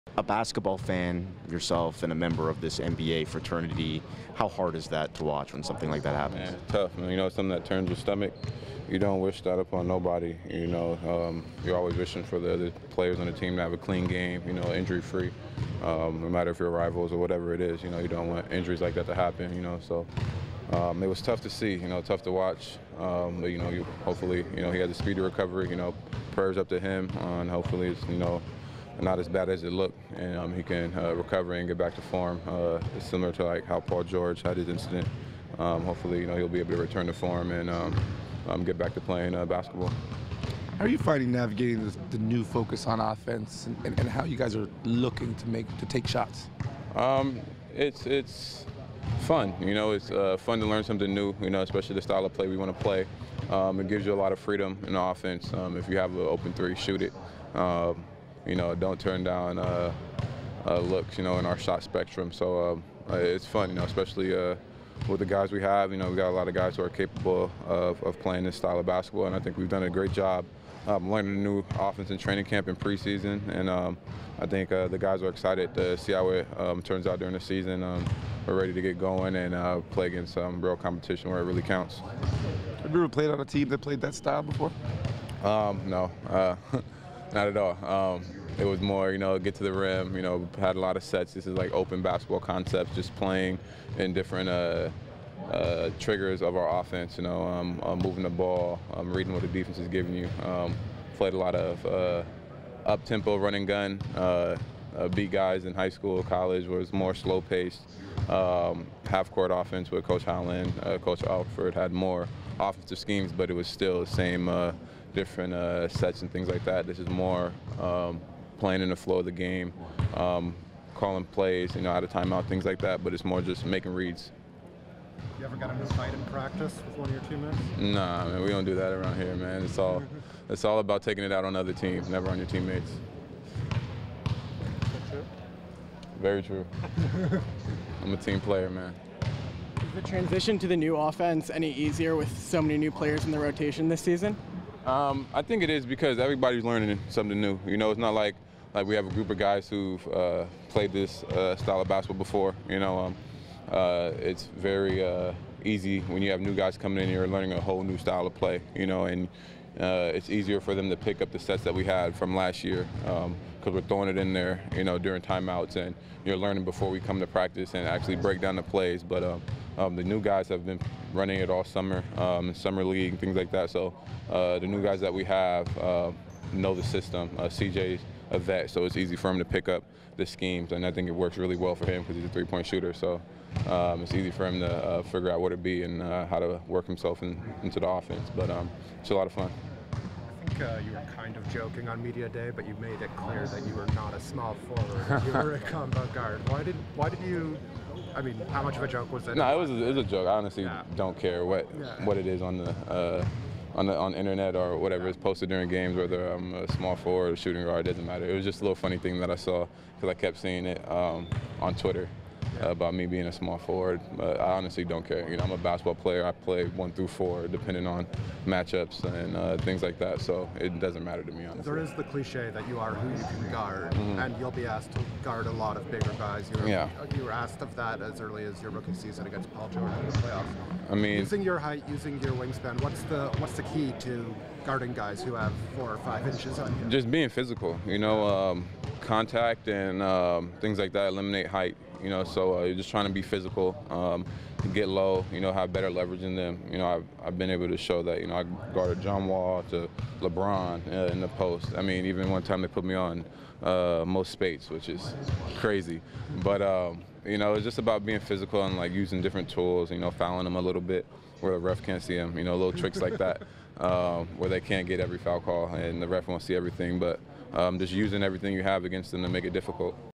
Raptors Practice: Norman Powell - October 18, 2017
Norman Powell meets with the media after Wednesday's practice, as the Raptors prepare to start the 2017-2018 season tomorrow against the Bulls.